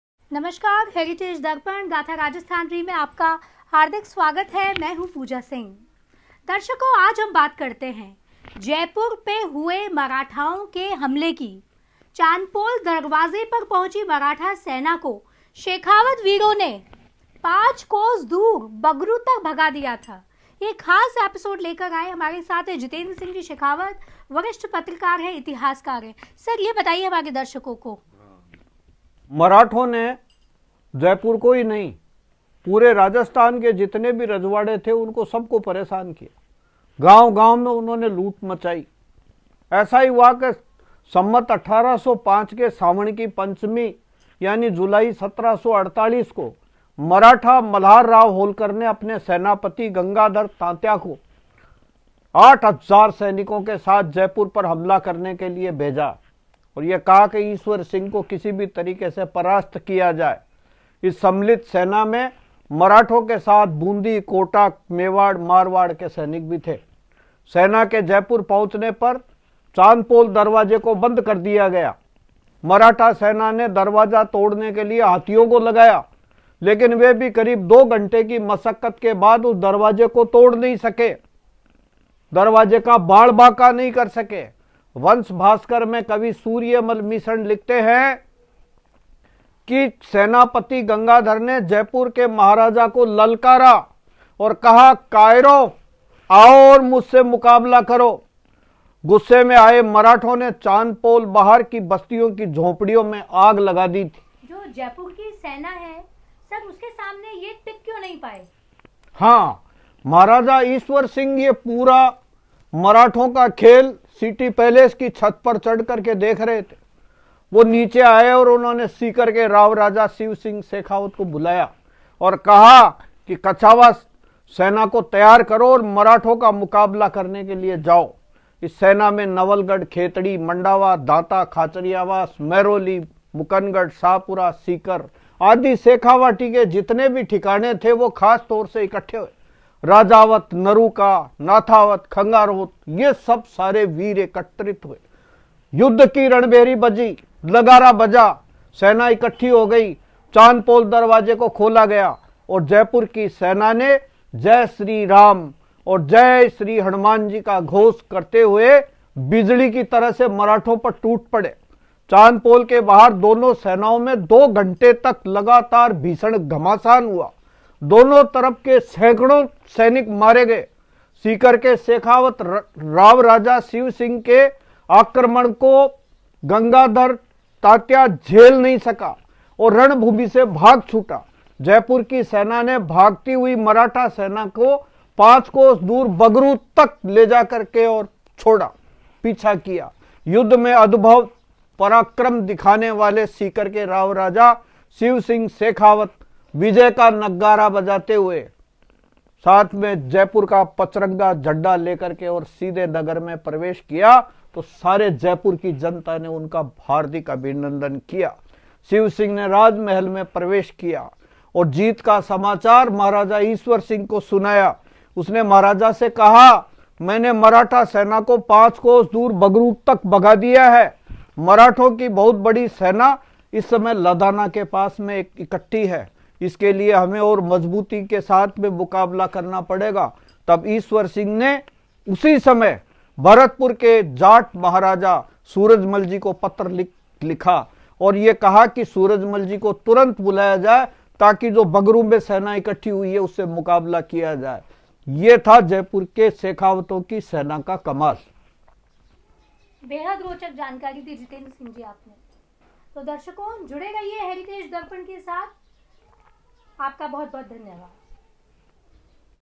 "शेखावत वीरों" ने मराठा सेना को जयपुर से बगरू तक "दौड़ा दौड़ा" कर भगा दिया था।
Panchami of Sawan of Samvat 1805 i.e. on July 1748, Maratha Malhar Rao Holkar sent eight thousand soldiers under the leadership of General Gangadhar Tatya to attack Jaipur Maharaja Ishwari Singh.
 The Marathas included soldiers from Bundi, Kota, Mewar and Marwar along with the Marathas.  The Chandpol door was closed after the army reached Jaipur